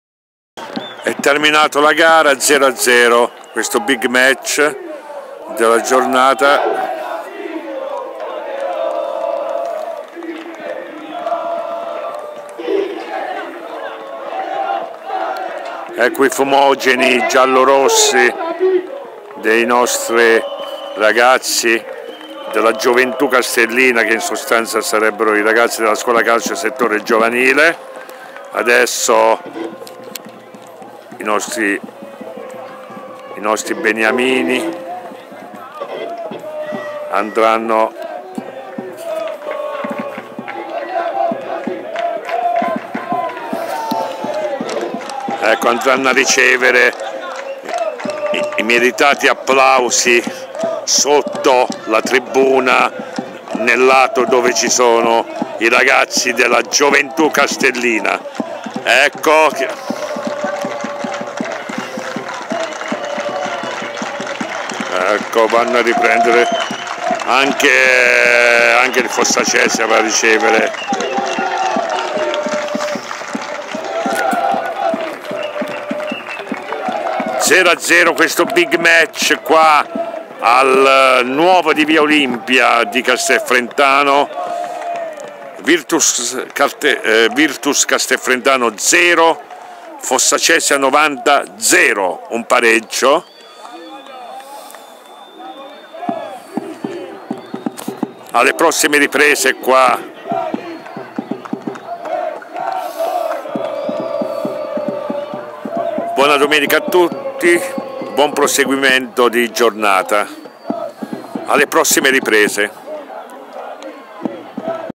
VIRTUS CASTEL FRENTANO " 2^CATEGORIA GIRONE E. Stagione 2018 - 2019
E' terminata la gara valevole per il Campionato di 2^ Categoria contro la Capolista Fossacesia 90 . Risultato di parità al termine dei 90 minuti regolamentari .